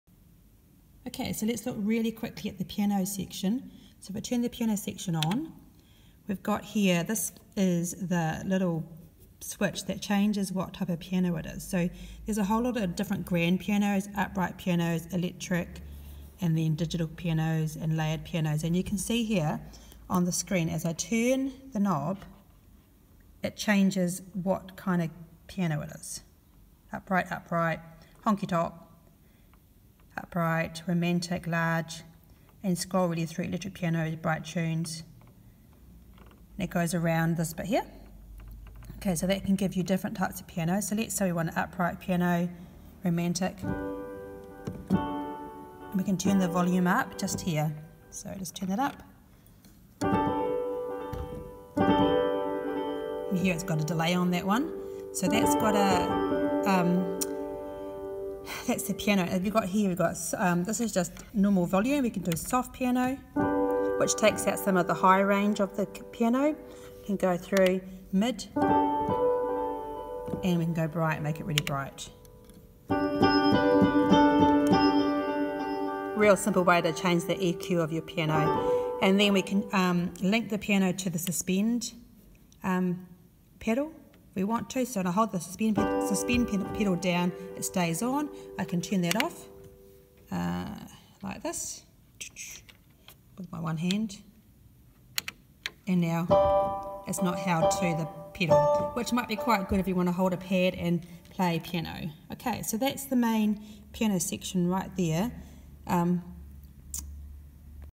NORD STAGE 3 piano tutorial
A very quick look at the piano section of the NORD